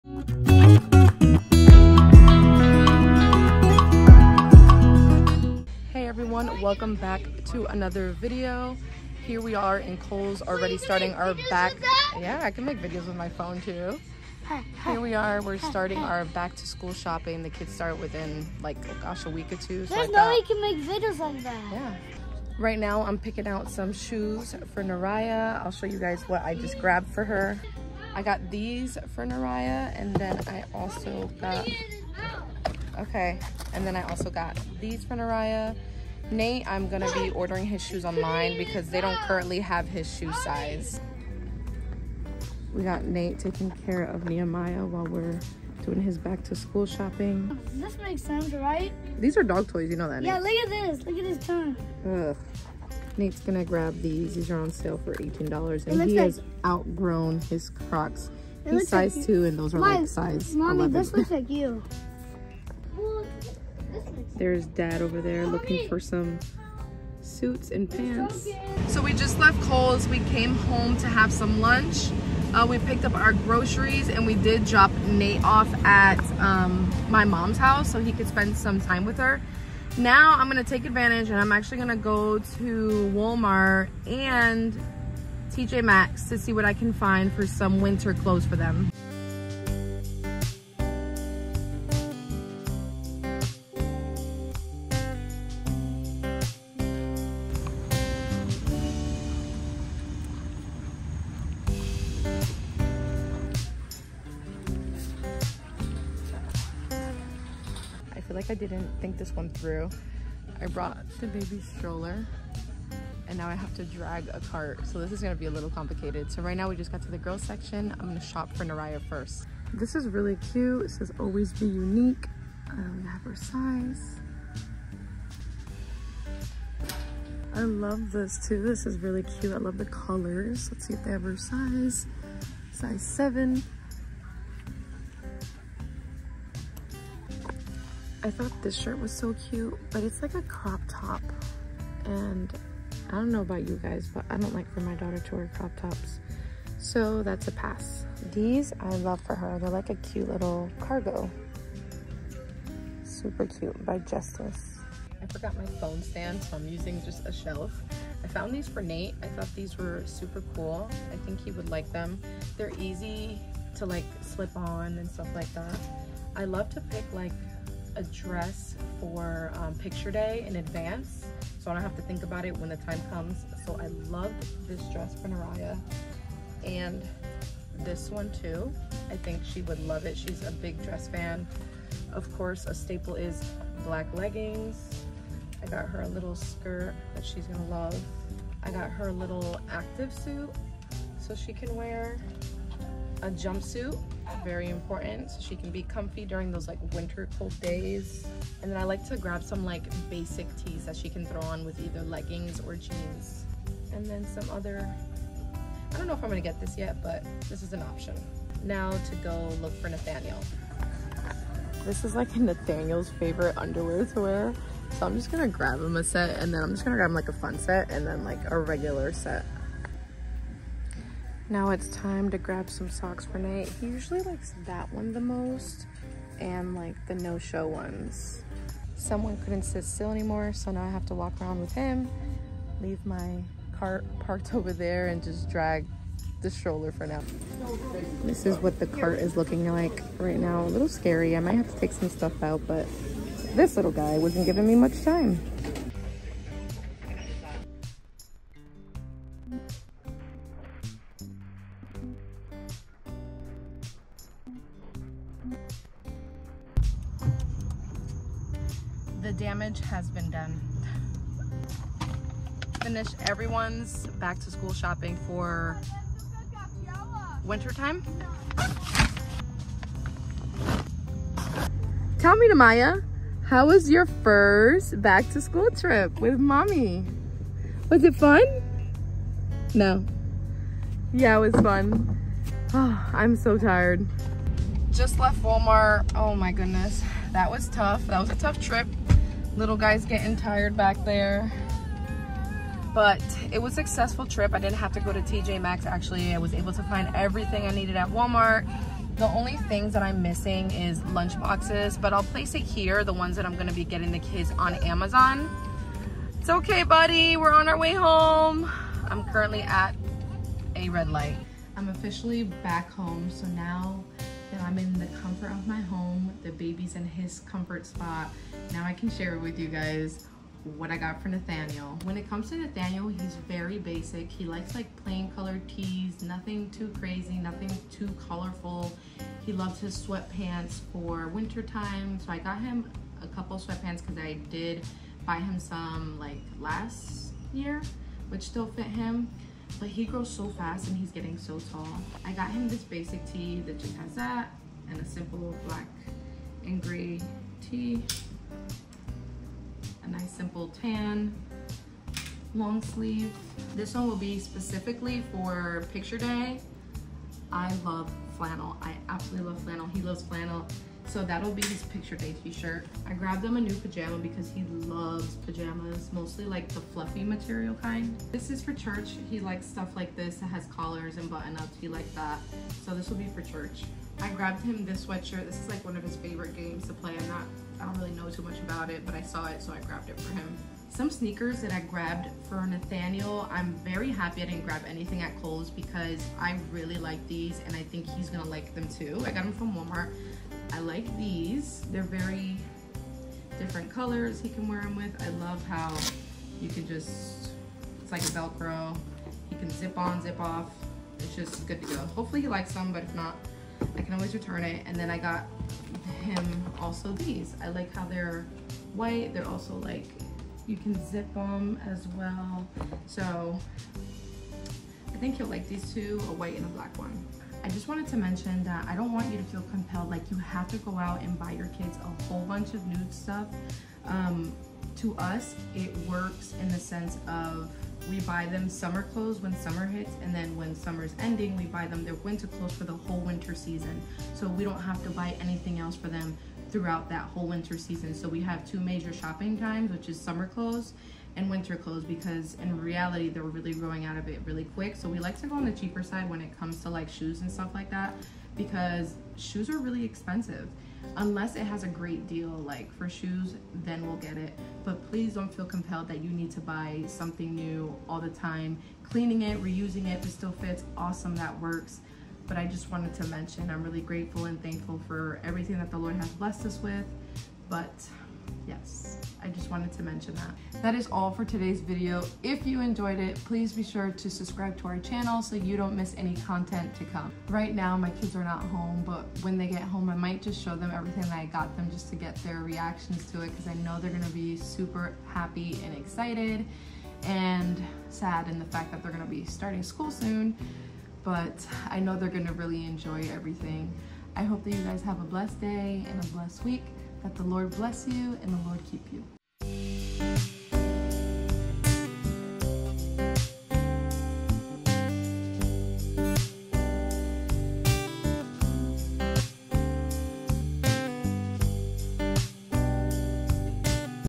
Back To School Shopping!
Hi! today we shopped for some back to school items for the littles! They sure are somewhat sad and excited to start school!